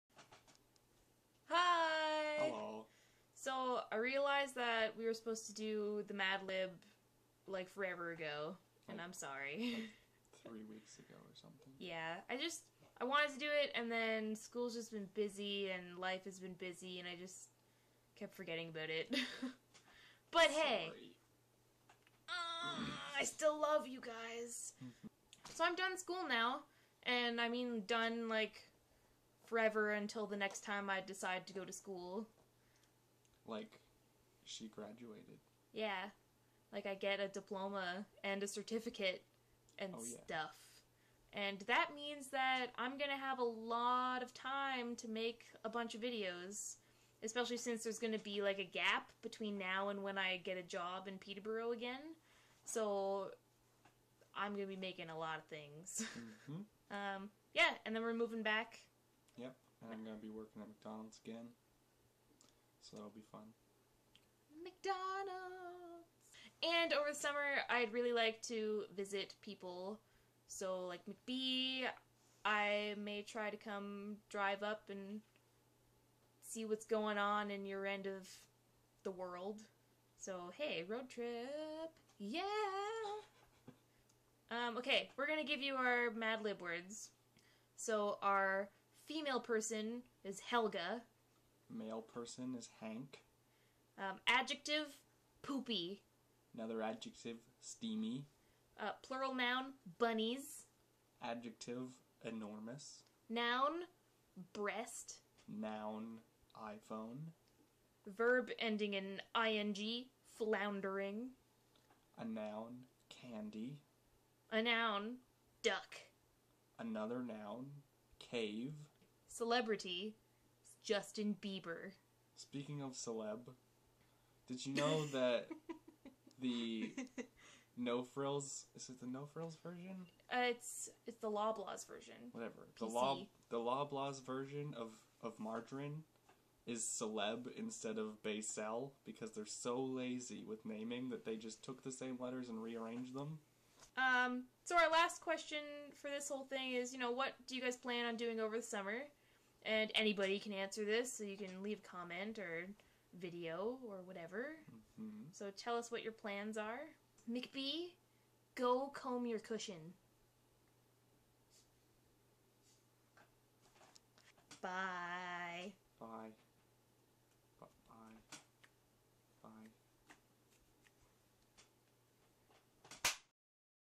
Madlib Words and updates [AB&NB]
Madlib words incase you missed them.
Helga, Hank, poopy, steamy, bunnies, enormous, breast, iPhone, floundering, candy, duck, cave, Justin Bieber.